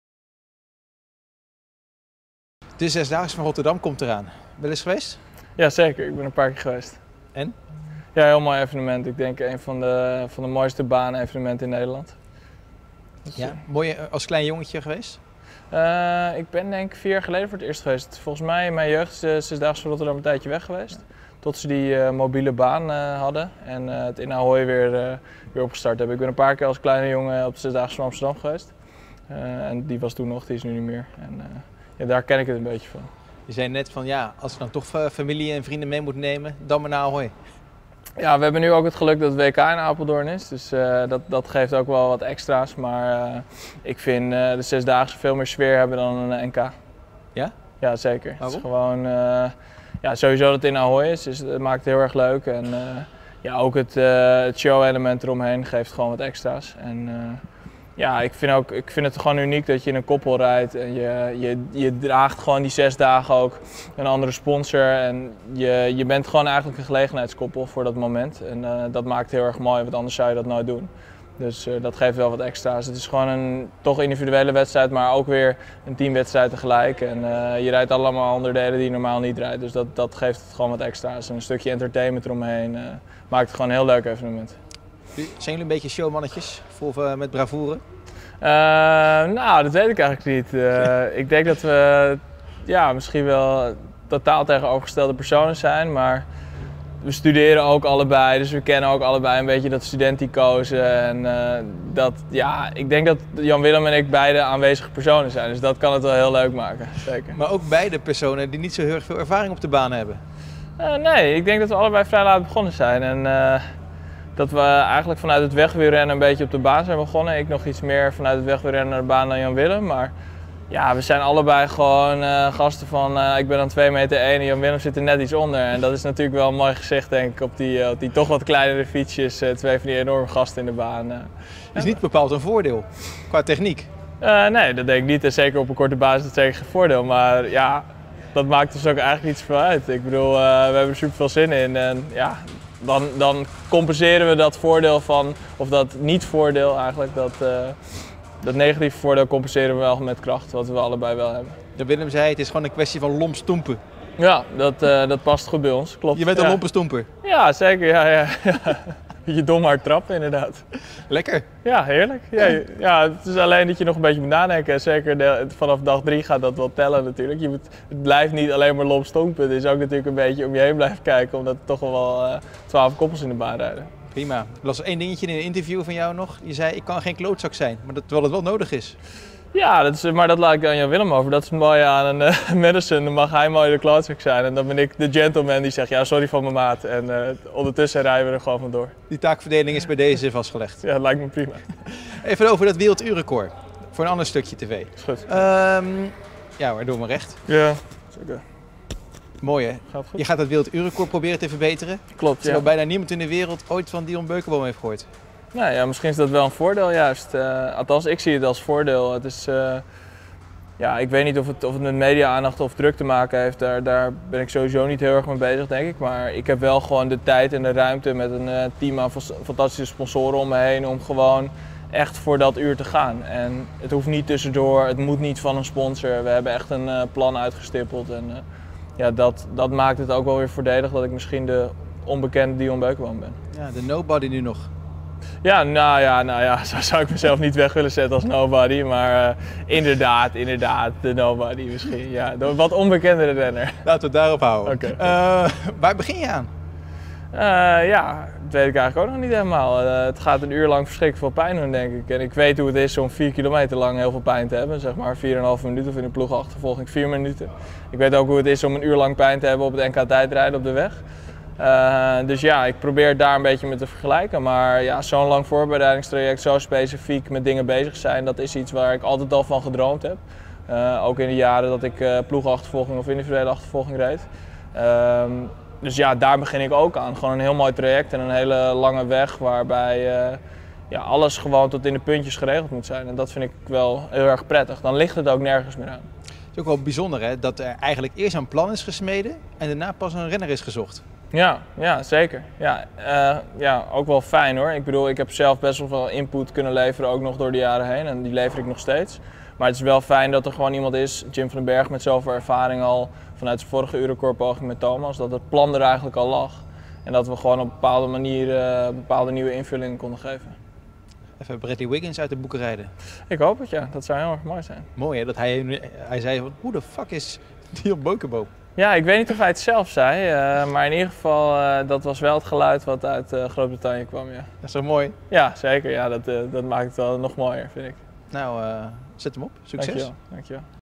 Dion Beukeboom over de komende Zesdaagse van Rotterdam
Het is nog twee weken wachten, maar dan gaat de Wooning Zesdaagse van Rotterdam in Ahoy van start. Bij dit baanwielerevenement rijdt de ploeg Delta Cycling Rotterdam een heuse thuiswedstrijd. Dion Beukeboom en Jan-Willem van Schip zullen namens de Rotterdamse formatie als duo op de fietsen zitten.De studenten zijn momenteel hard in training om tijdens de Zesdaagse goed voor de dag te komen. Beukeboom en Van Schip maken namelijk hun debuut in dit aanstaande spektakelstuk in het baanwielrennen. &quot;Het heeft iets mythisch&quot;, aldus Van Schip. Ook Beukeboom kan niet wachten tot de Zesdaagse begint: &quot;Sowieso dat het evenement in Ahoy is, maakt het erg leuk. Het showelement geeft wat extra&apos;s.&quot;Beukeboom en Van Schip weten zelf dat ze niet als favorieten aan de Zesdaagse zullen starten. Verder zijn beide heren in Rotterdamse dienst lang van stuk. &quot;Ik denk dat het wel een mooi gezicht is als twee van die grote gasten op die toch wat kleinere fietsjes zitten&quot;, lacht Beukeboom. Van Schip zegt dat hij samen met z&apos;n teamgenoot het vooral van hard trappen moeten hebben: &quot;Als we met z&apos;n allen lomp gaan stampen, dan komen we wel een eind.&quot;